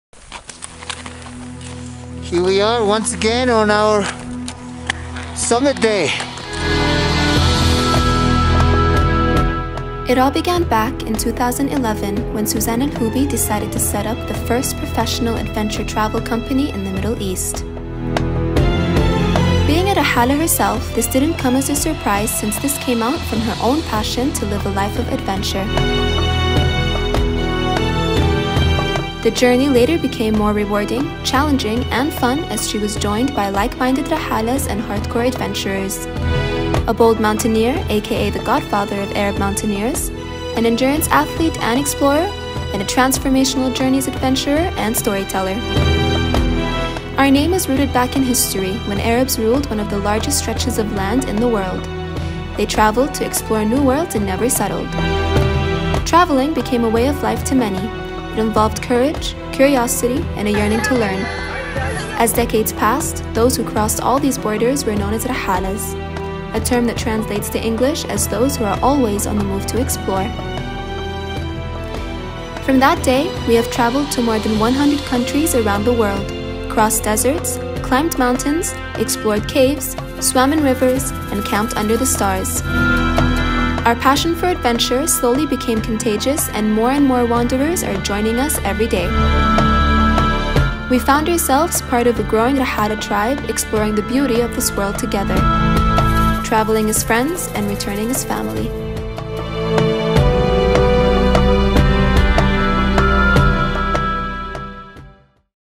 RAHHALAH: OUR STORY
It all began back in 2011 when Suzanne Al Houby decided to set up the first professional adventure travel company in the Middle East, and ever since then, we’re always on the move to explore!